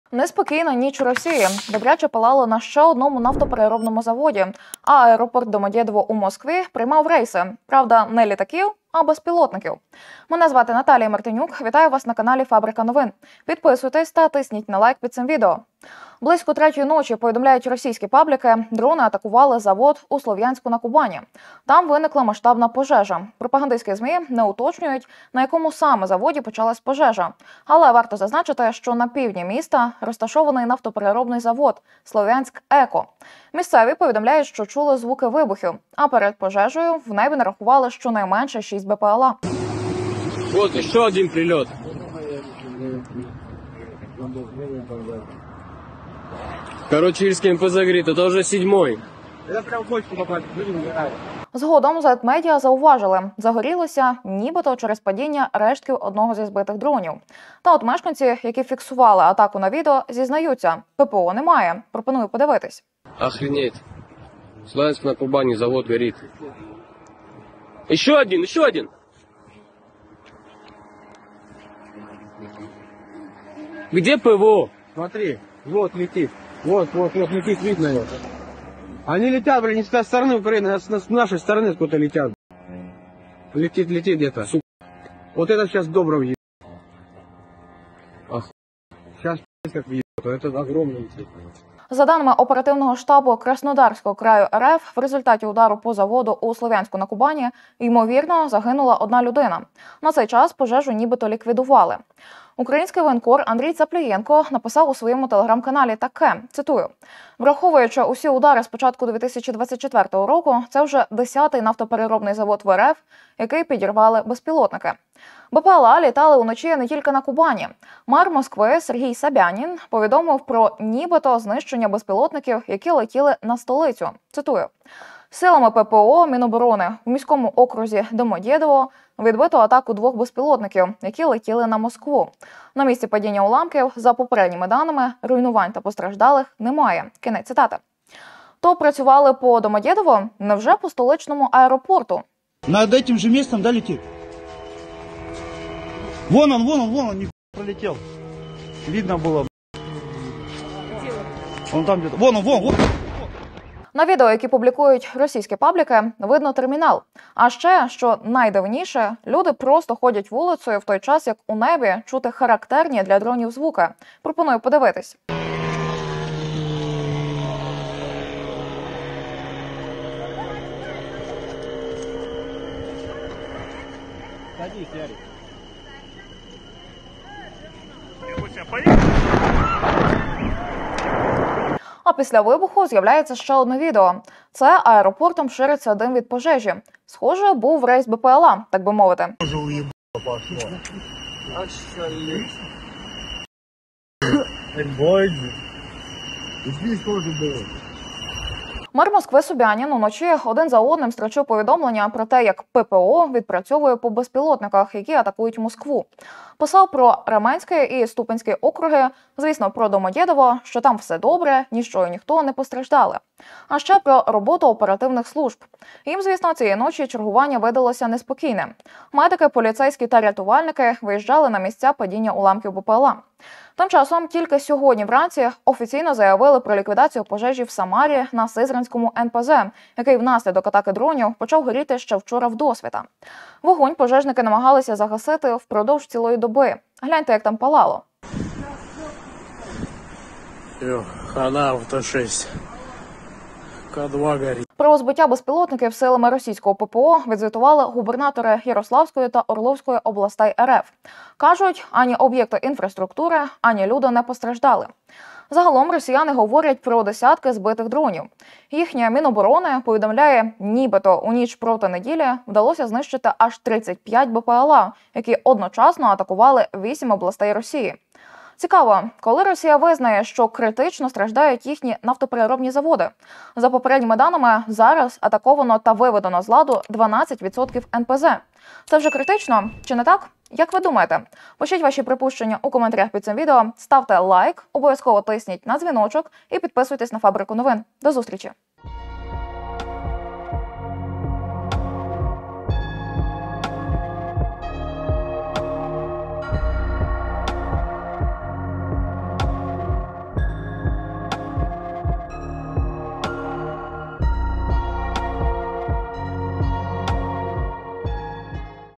💣МОСКВУ АТАКУВАЛИ! З аеропорту валить дим. На Кубані ВИБУХНУВ ЗАВОД, 7 прильотів в одну точку
На Росію запустили дрони! Зокрема, атакували завод у Слов'янську-на-Кубані. Там виникла масштабна пожежа. Місцеві повідомляють, що чули звуки вибухів, у небі нарахували щонайменше 6 БПЛА. Крім того, мер Москви Сергій Собянін повідомив про нібито знищення безпілотників, які летіли на столицю. У Мережі припустили, що дрони летіли на аеропорт Домодєдово. На відео, які публікують роспабліки - видно термінал, а також дим. Міноборони РФ повідомляє, нібито у ніч проти неділі, вдалося знищити аж 35 БПЛА, які одночасно атакували 8 областей Росії.